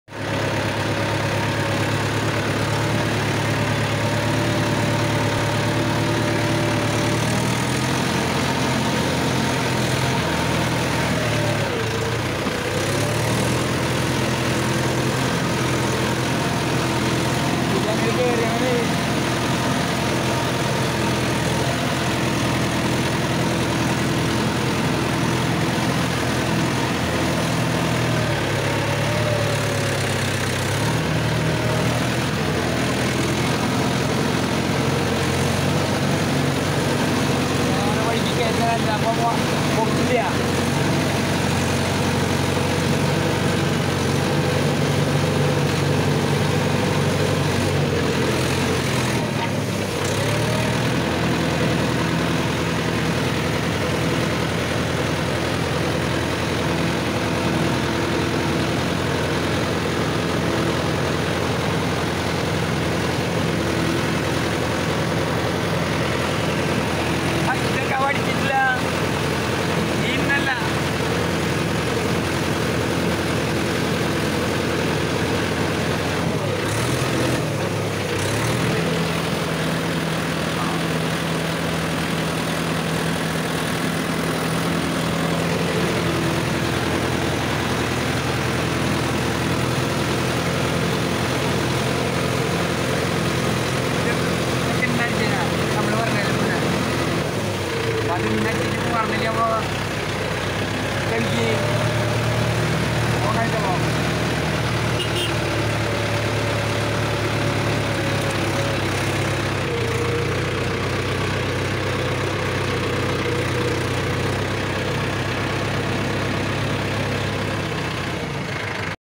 Mahindra yuvo 575 di 4 wd TRACTOR cabin view
Mahindra yuvo 575 si 4 wd tractor running at field.cabin view